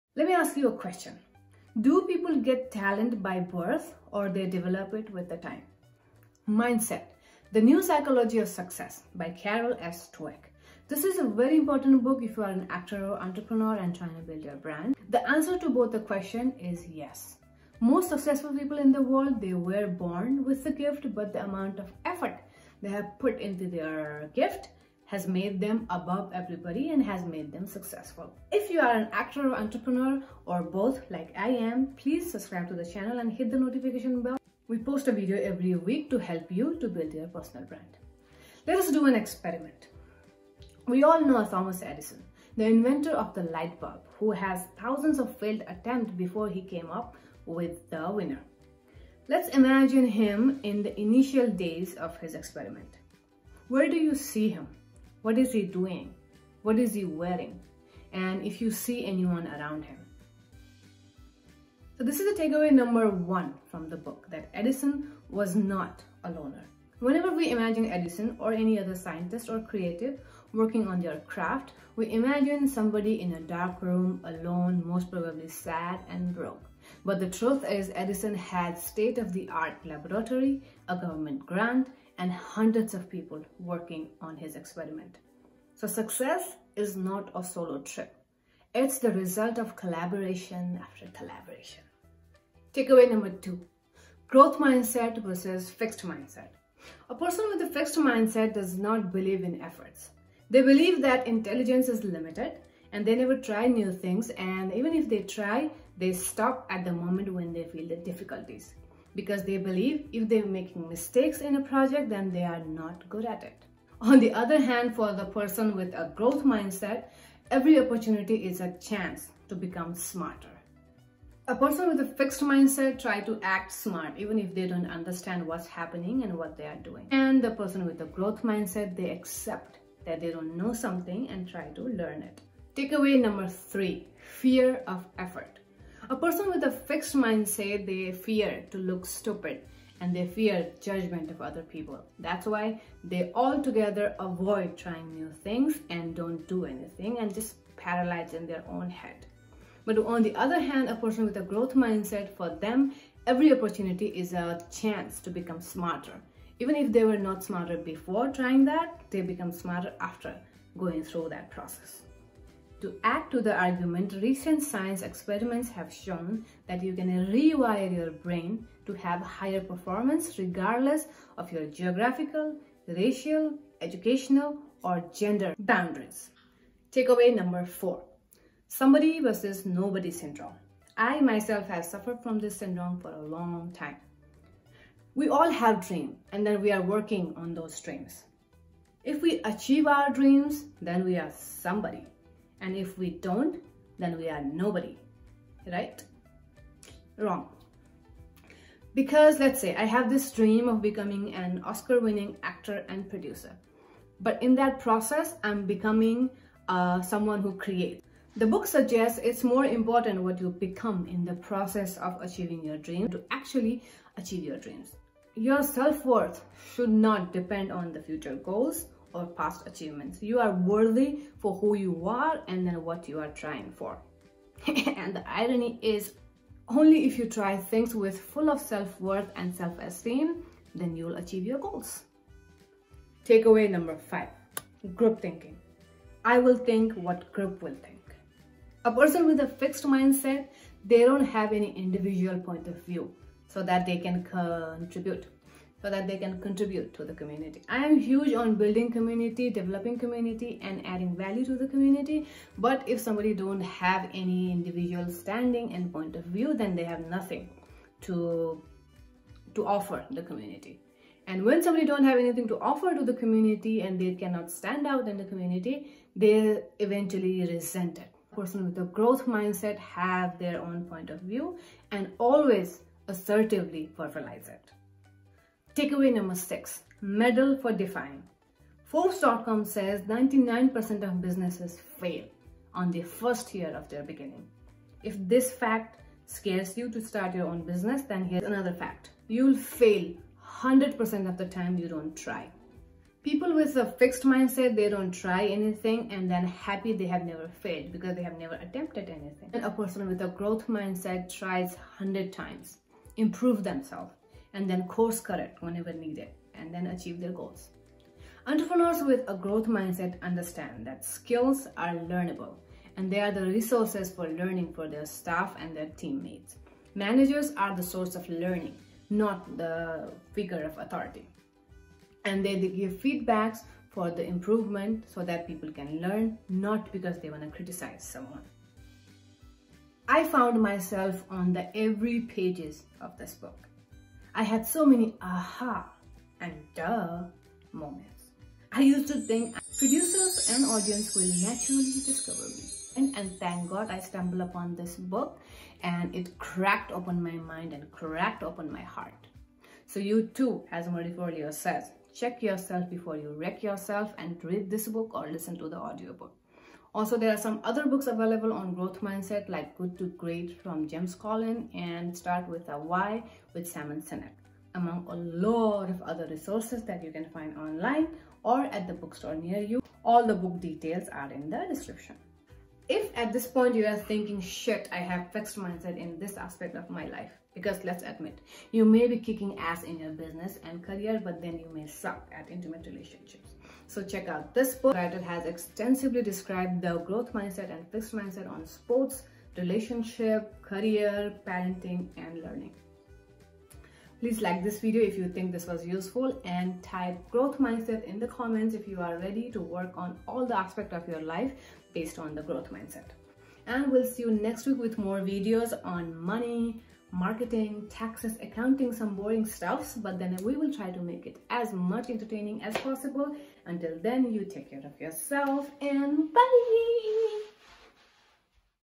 Mindset By carol Dweck | Summary | For Entrepreneurs And Actors
Researches have shown, most of the acting school graduated actors, especially the toppers, struggle to make it as a successful actor, because they leave school with a fixed mindset, that they have learned whatever they needed to learn in order to make it into the world of acting.
The similar principle applies to entrepreneurs

I am not saying training in acting is not needed, that’s the last thing I would ever say, but we actors need to understand learning doesn’t end with the degree we had, ten years ago.
I highly recommend this book, regardless of what phase you are as an actor or entrepreneur. Reading this book will set you on the path to becoming an infinite learner. I am going to talk about some of the key concepts from the book today.
# Growth mindset vs Fix mindset

Mindset By Carol Dwek| Summary for actors and entrepreneurs